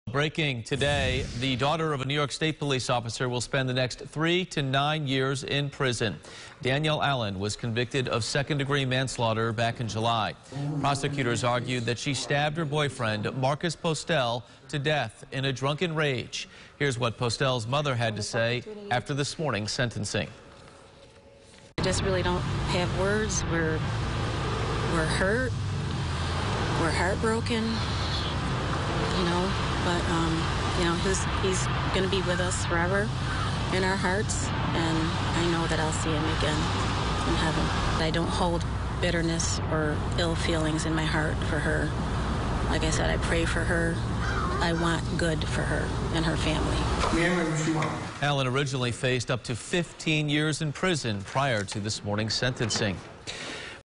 Livingston Co. woman gets 3 to 9 years in prison for boyfriend's death
A Livingston County woman has been sentenced to three to nine years behind bars for the death of her boyfriend in November 2016.